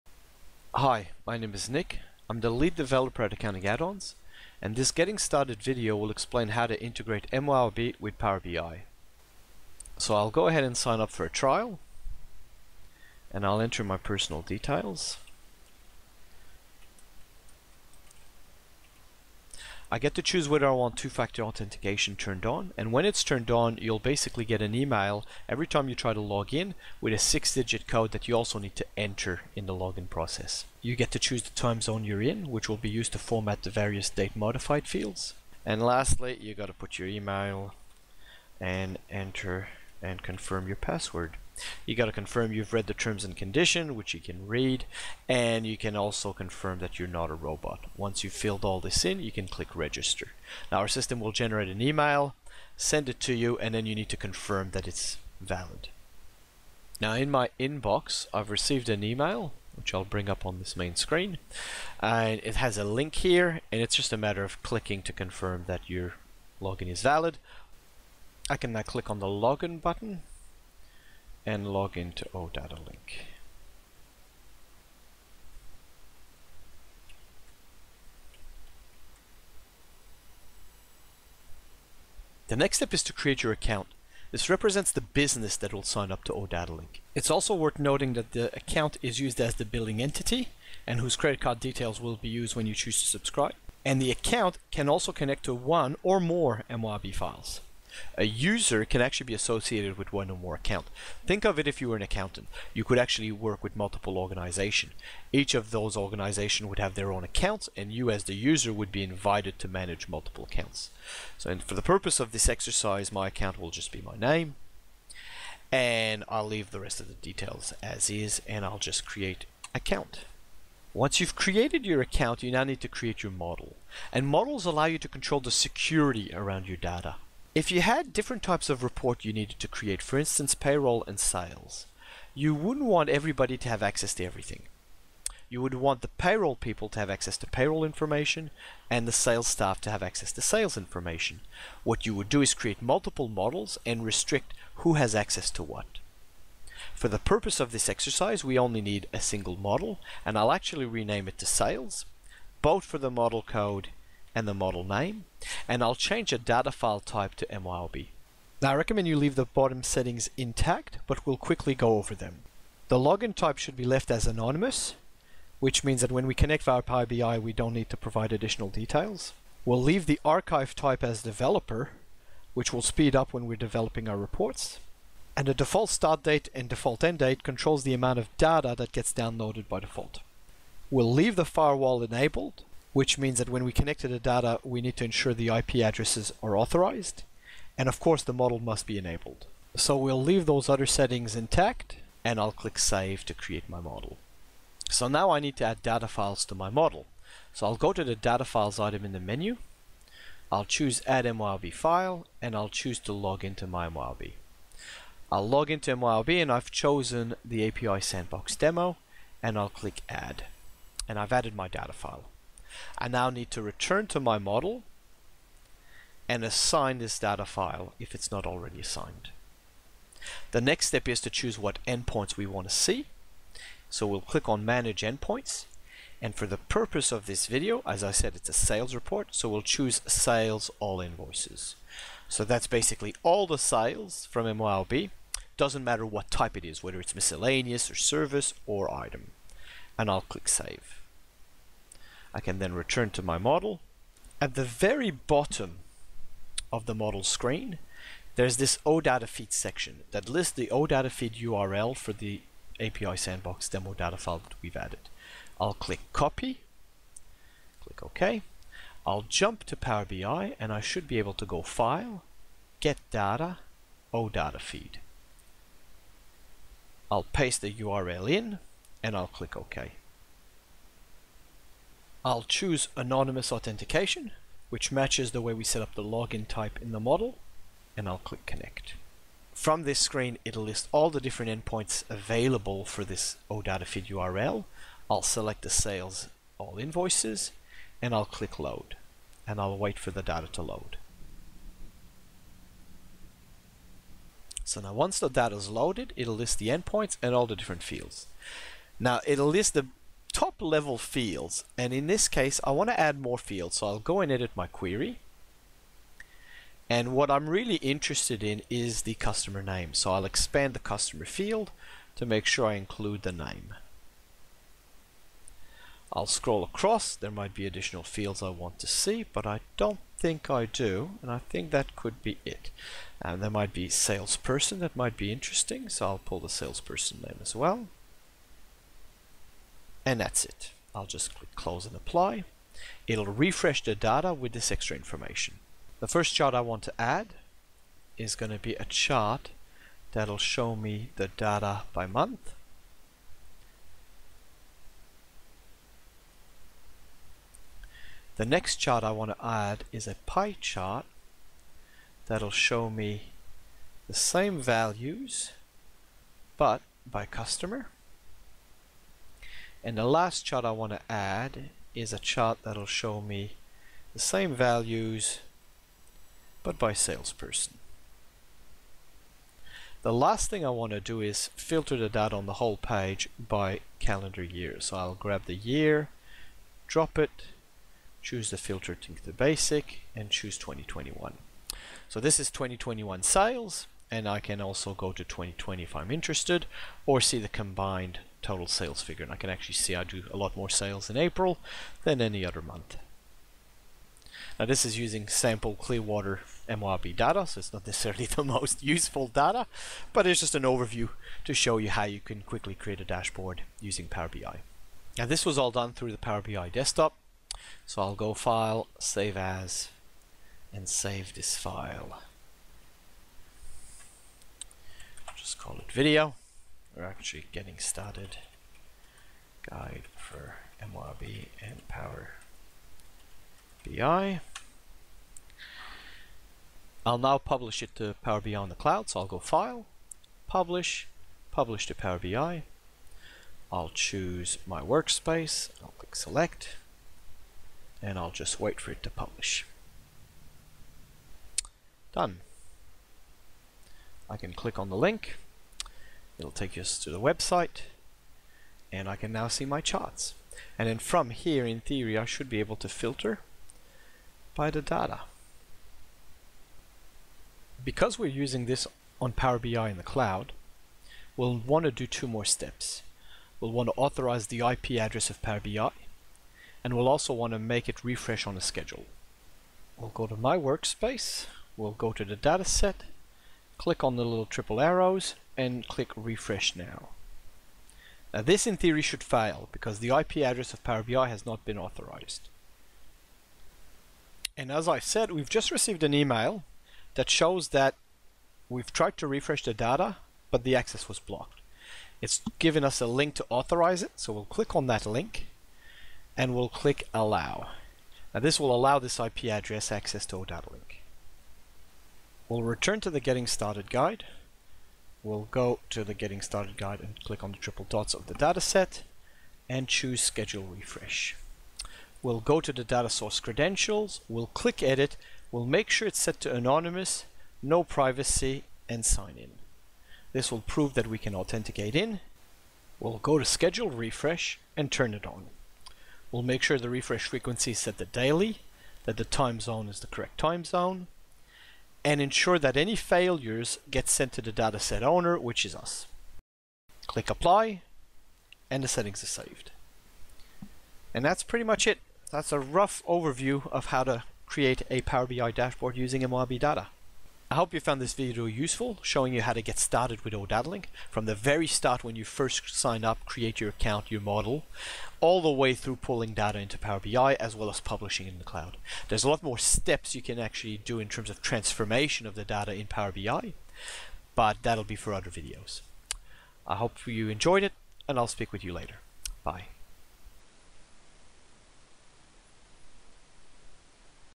Getting Started Guide for Power BI and MYOB AccountRight Live / MYOB Essentials - OdataLink
OdataLink is an OData feed provider that shapes the MYOB AccountRight Live and MYOB Essentials data into a format that can readily be used by Power BI without the need for any external software, additional setup or complex programming.

It works with both the desktop and cloud versions of Power BI.

This Getting Started Guide will cover everything you need to know to start integrating MYOB with Power BI: from setting up your account to pulling the data into Power BI.